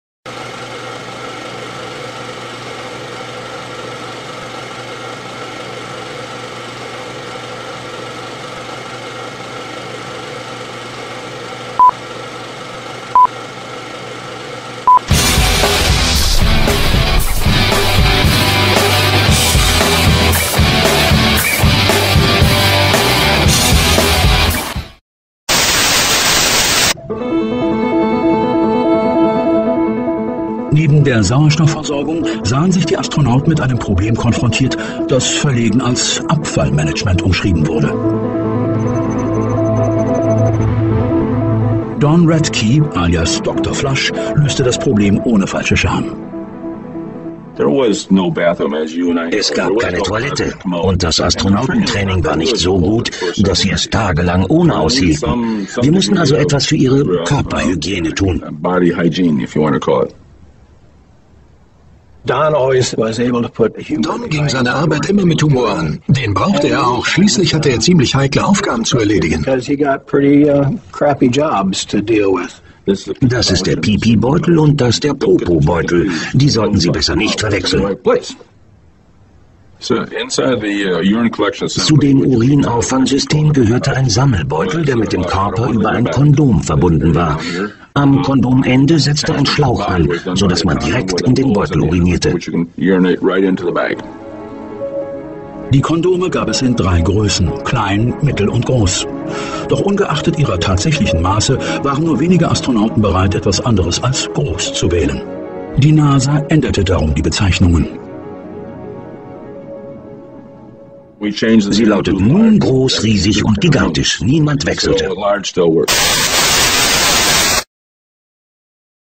ReEdit - Nasa enthüllt [ RealSatire ]
Quelle ZDF, ARD, WELT. irgend son Qualitätssender, is halt lustig, was die NASA so "enthüllt"

Hier ist denn noch ein praktisches Anwendungsbeispiel für den Pipibeutel, nicht zu verwechseln mit dem Popobeutel:
...ich wette, er hat auch "Groß" genommen...